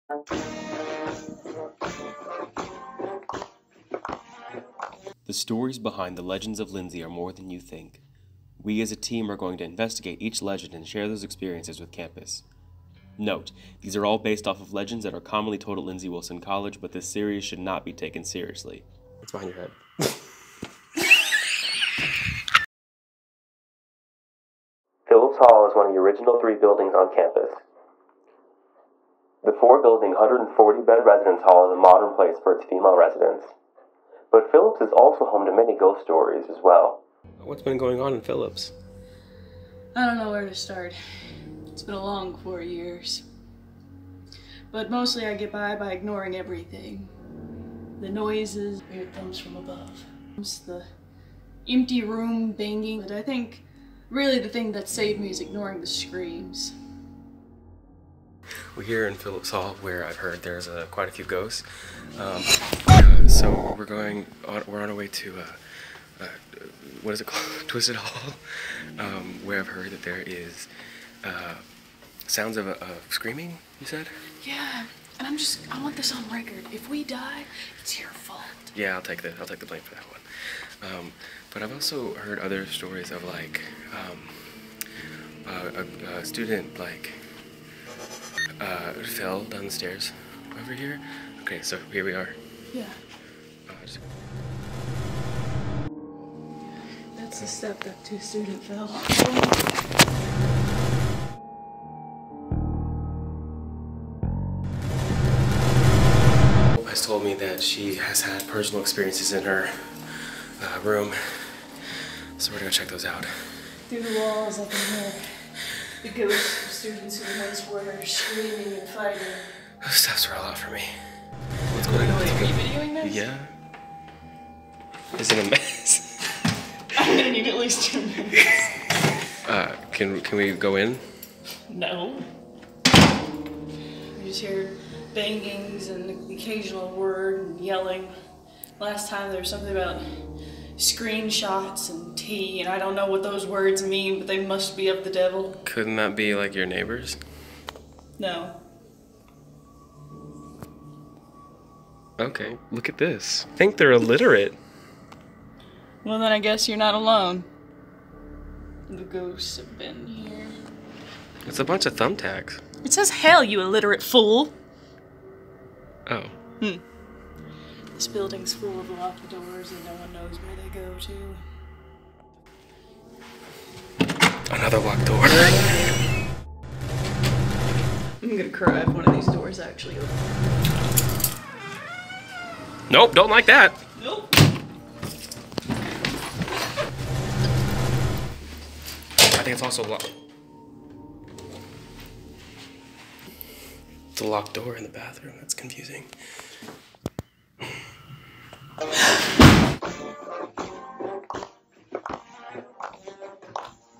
Lindsey Legends Phillips Hall
A friend and I decided to take on the many legends of Lindsey Wilson College, in this episode, we talk about the haunting of Phillips hall